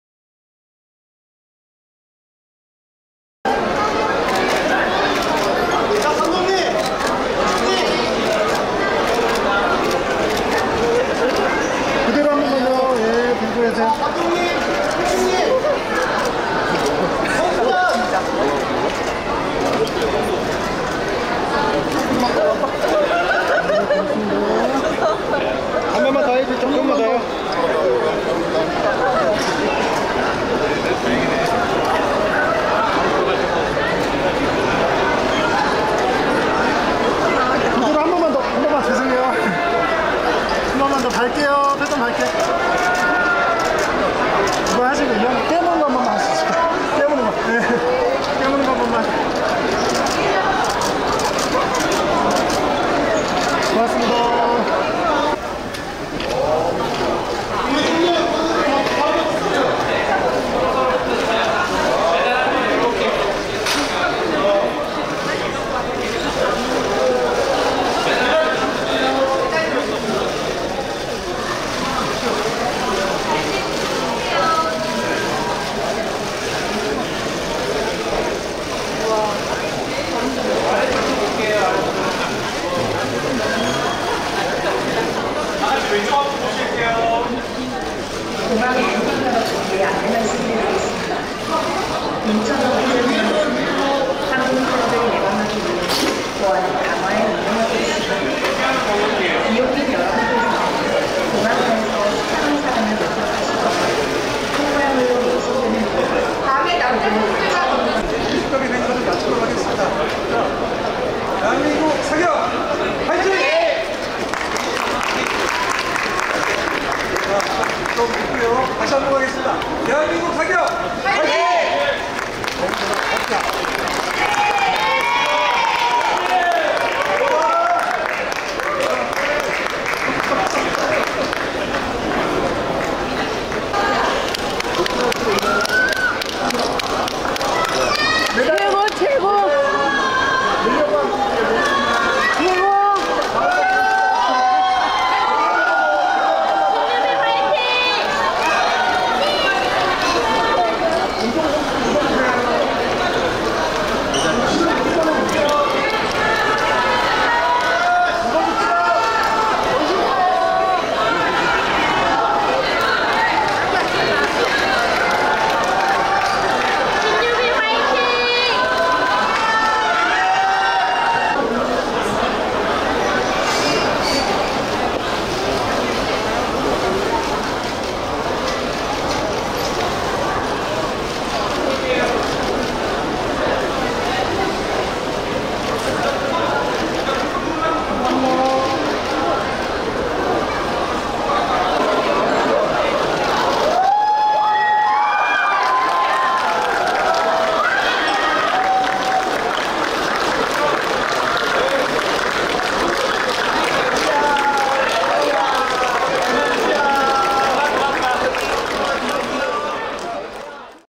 "이 장면, 놓치면 후회합니다!" 파리 올림픽 영웅들의 '금의환향' 모습 하이라이트ㅣ2024년 8월 20일
2024 파리 올림픽 메달리스트들의 귀국 현장을 취재한 영상을 재구성하였다.

촬영 및 편집 : 고초록 기자(스포테이너즈)